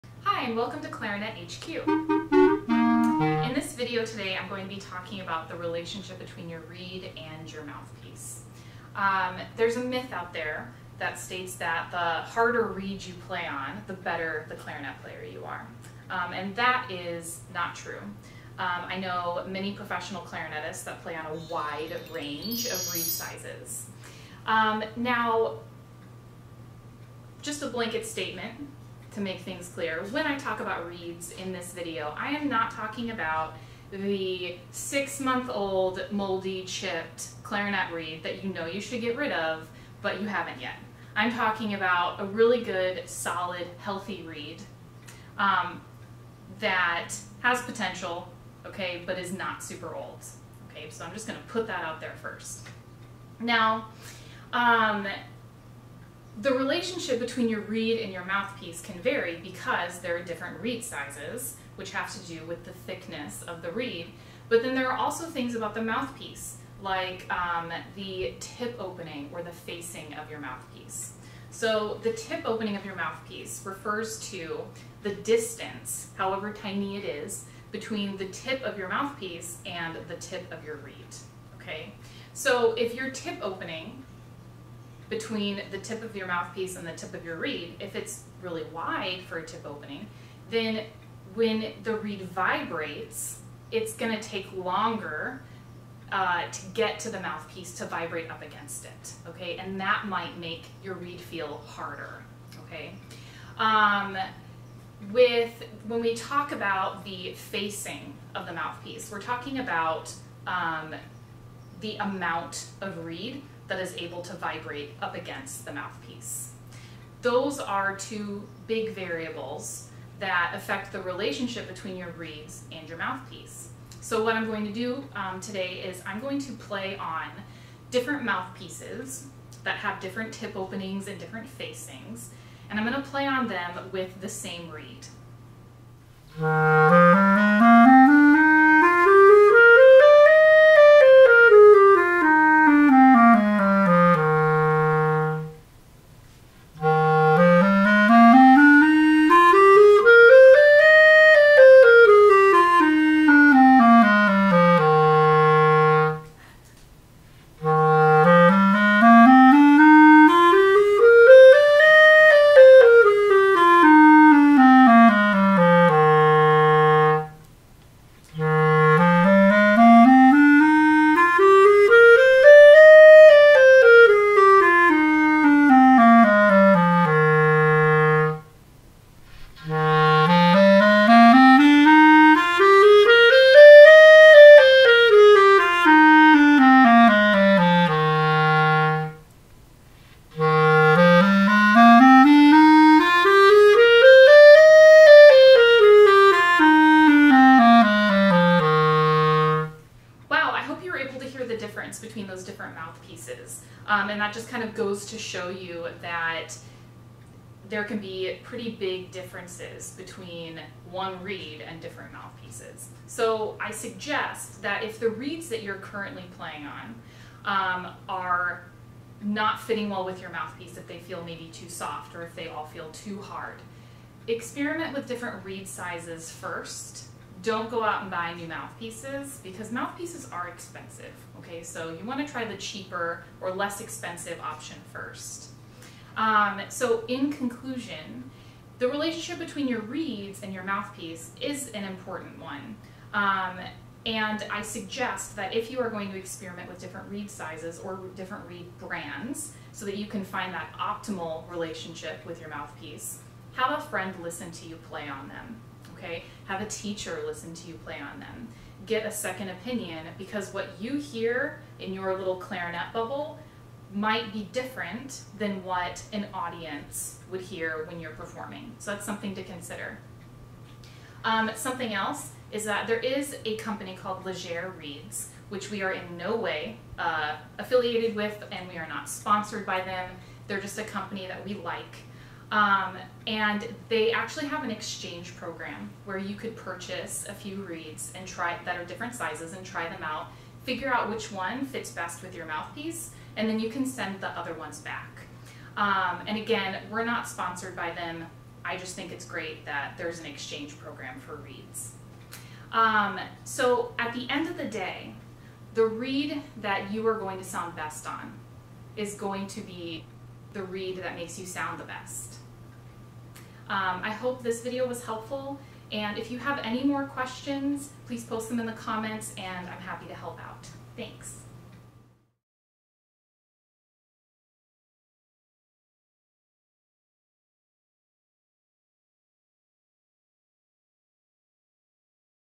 Finding the Best Reed for Your Mouthpiece
There are so many reed choices out there. How do you pick the best one for you? We'll give you some advice and bust some clarinet myths at the same time.